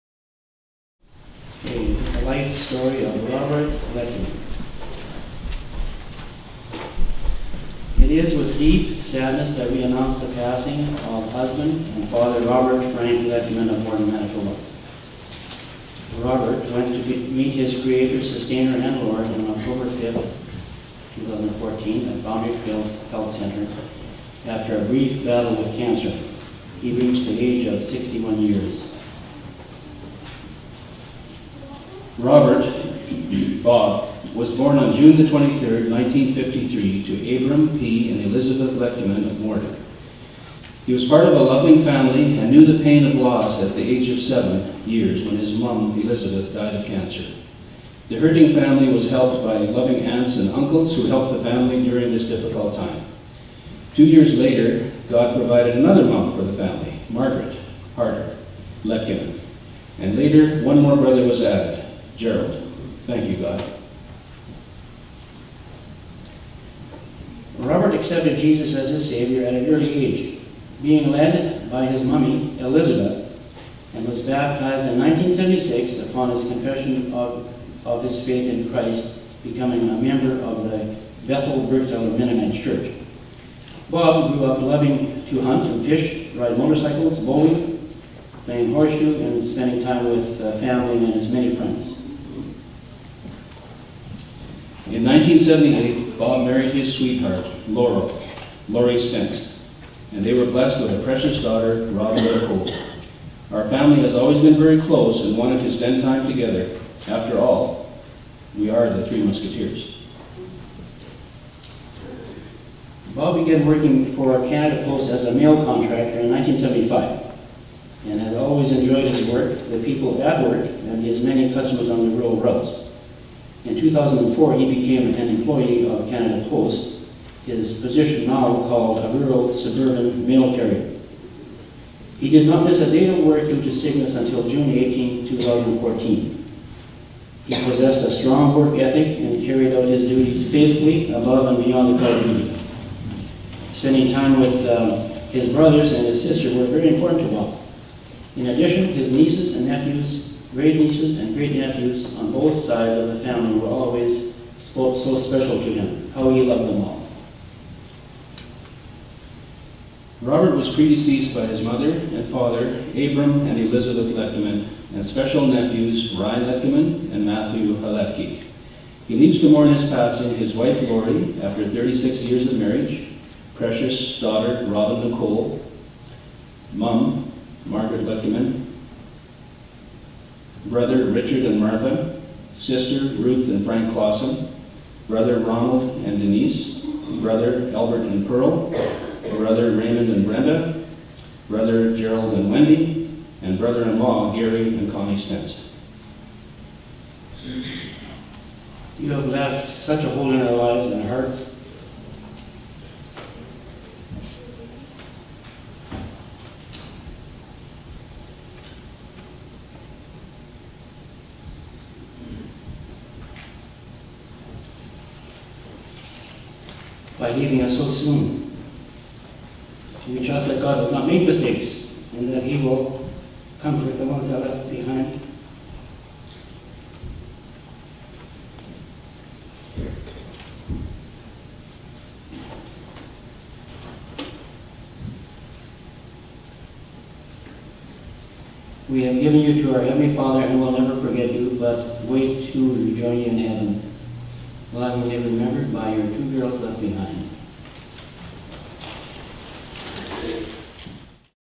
The Life Story of Bob Letkeman
As read at the funeral service by Albert and Ray Letkeman.